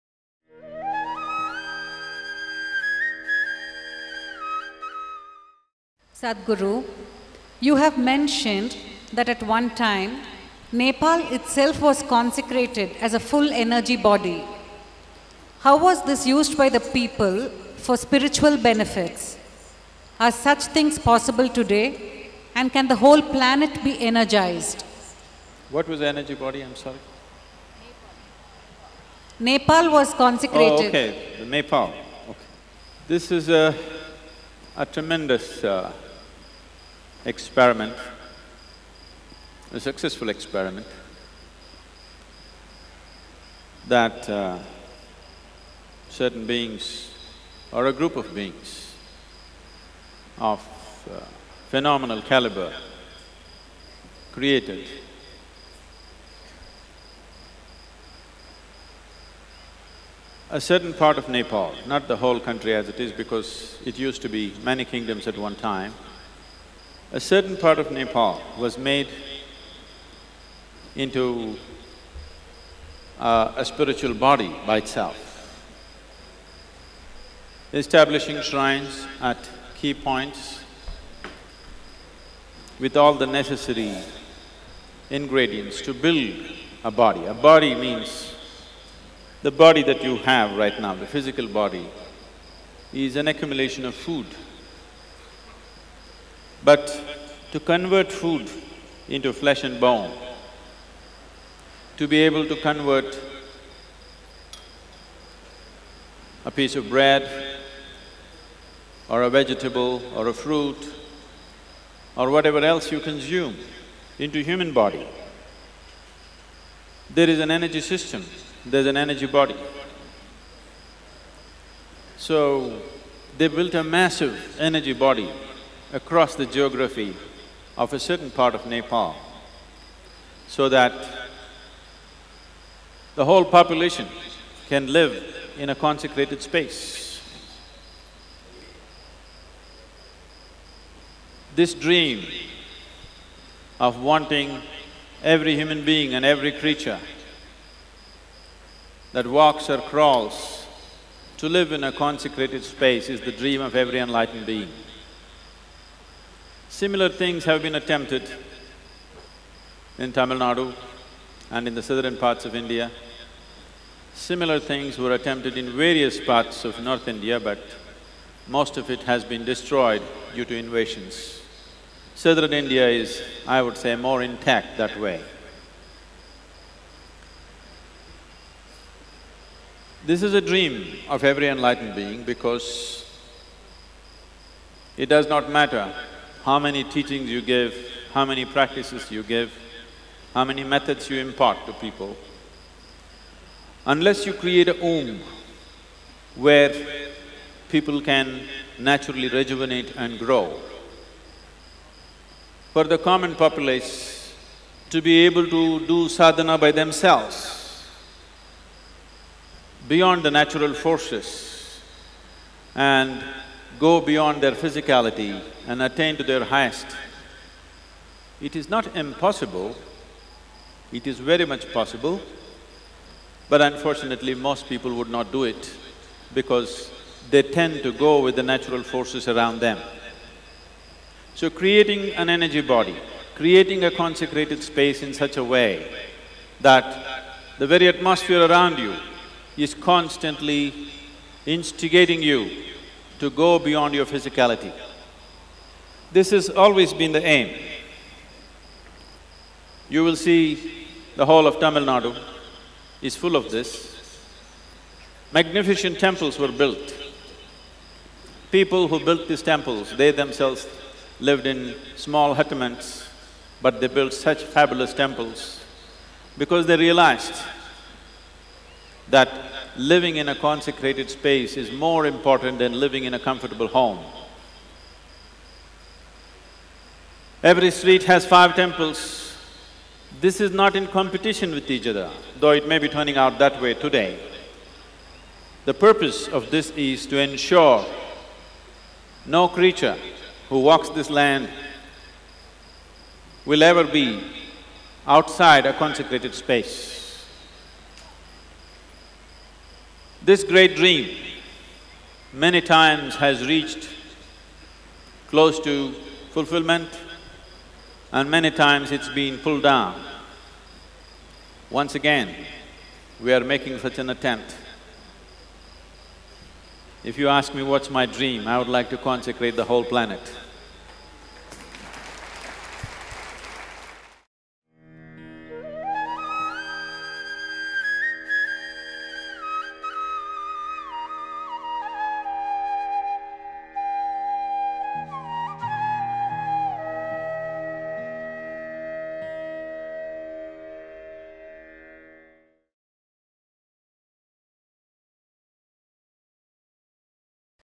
Nepal: A Living, Tantric Body | Sadhguru
Sadhguru explains how a certain part of Nepal was made into a spiritual body by itself, by establishing shrines at key points with all the necessary ingredients to build an energy body. This was done by a group of being s of phenomenal caliber so that the whole population could live in a consecrated space.